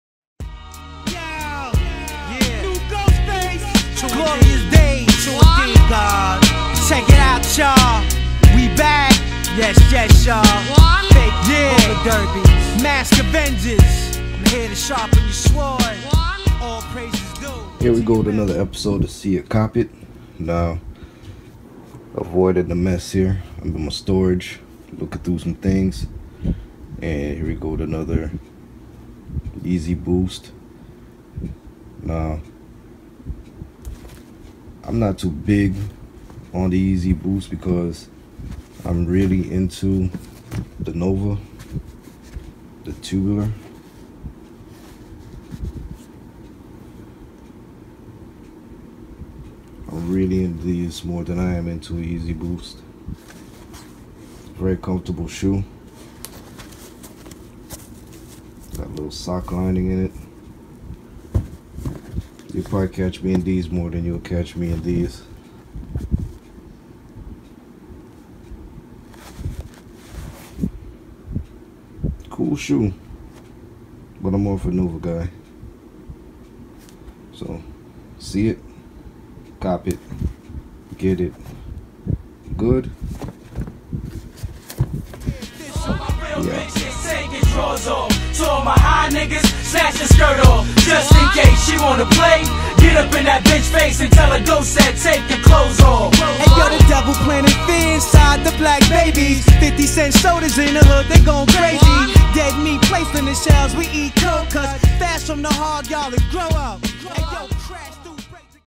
Adidas Yeezy Boost 350 V2 Green vs Tubular Nova
This episode of seeitcopit i explain why i'm more a fan of the Tubular than the YeezyBoost. Both are very comfortable sneakers but for me the Tubular Nova is more aesthetically pleasing than the Yeezyboost 350. A lot of the Hype driven kanye fan base may say otherwise but to each is own and my own no bias choice is the Tubular Nova.